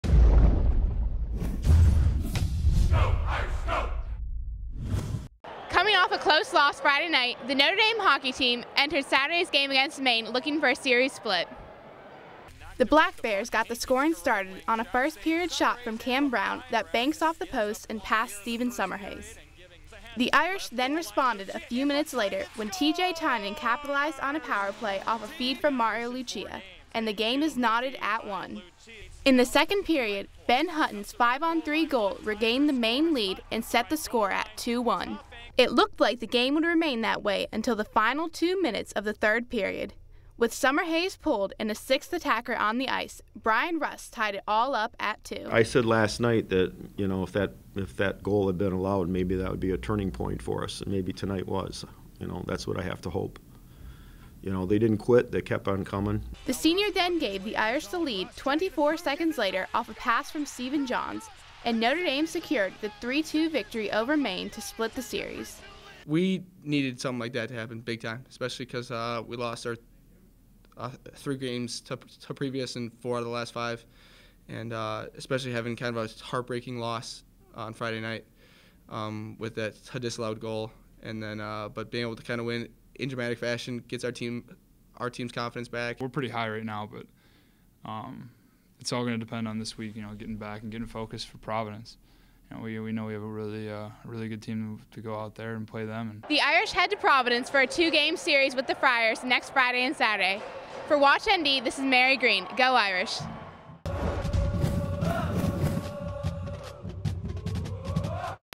Irish 3, Black Bears 2 - Notre Dame Hockey
The Notre Dame hockey team won a thriller on Saturday night, 3-2, to earn a series split with Maine.